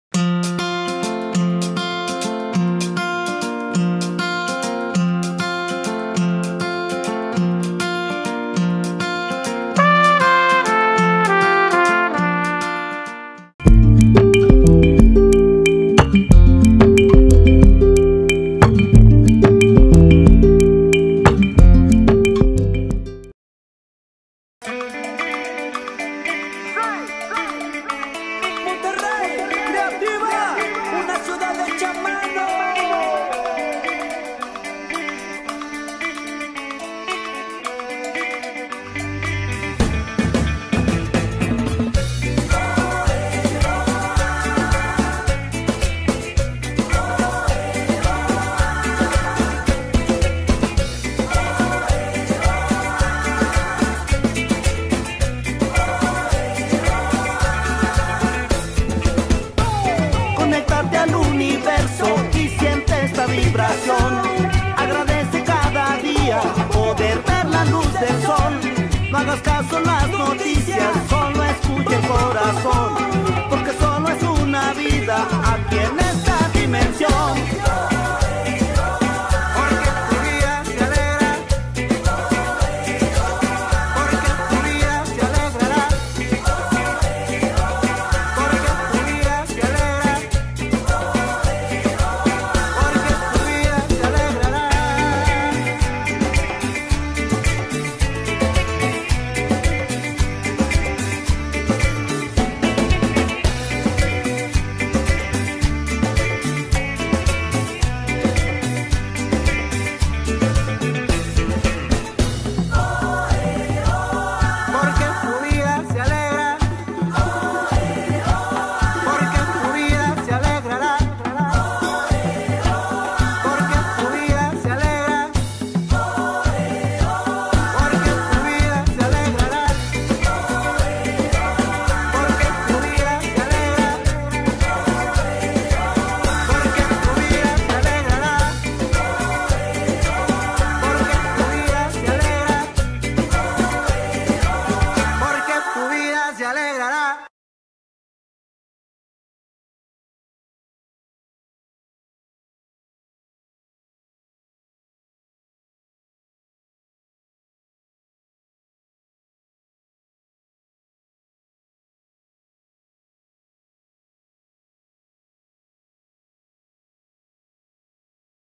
Resumen VII Jornadas Internacionales Ciudades Creativas Kreanta-Monterrey Creativa
Resumen de la séptima edición de las Jornadas Internacionales Ciudades Creativas Kreanta-Monterrey Creativa, cleberadas en la ciudad mexicana del 24 al 26 de octubre de 2014.